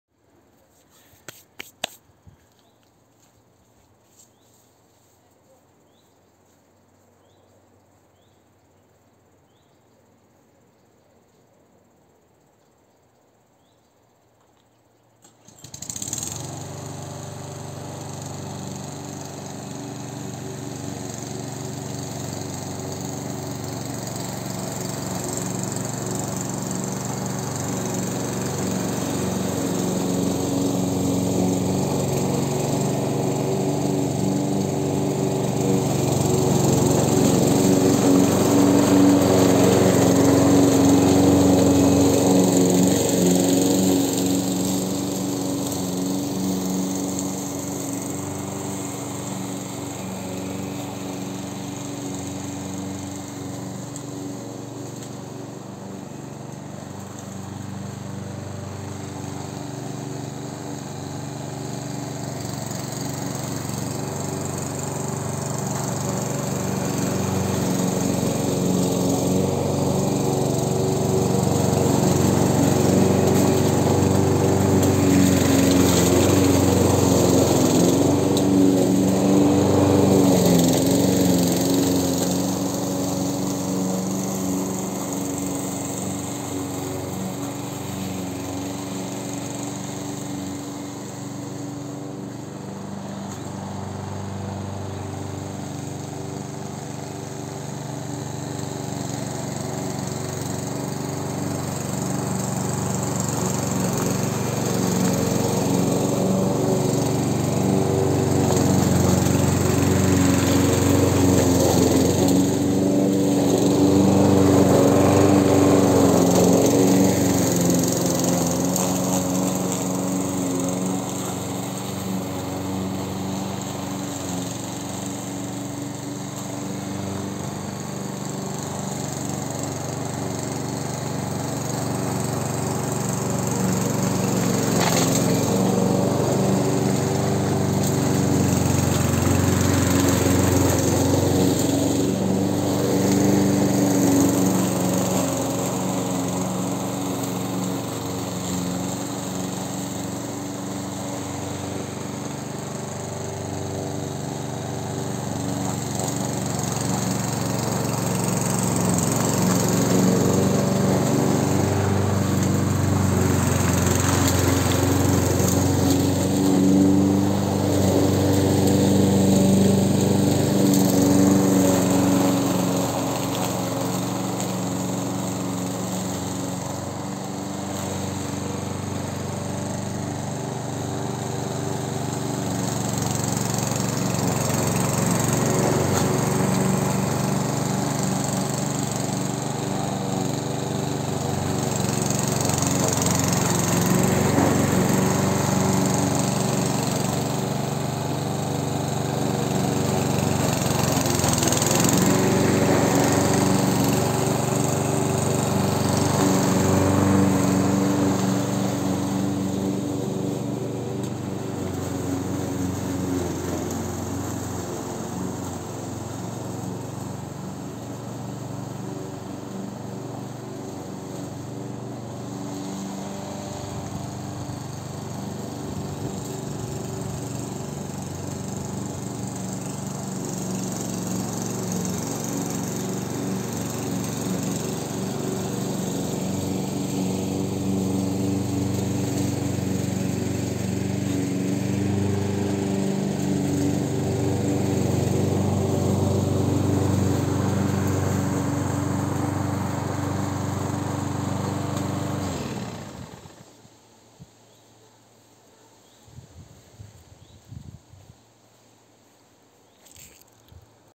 Žolės pjovimas benzinine I dalis (2020.06.27)
Filmuota su "Redmi Note 8Pro", 4K raiška.
Žoliapjovė "Amber line-B class 4in1".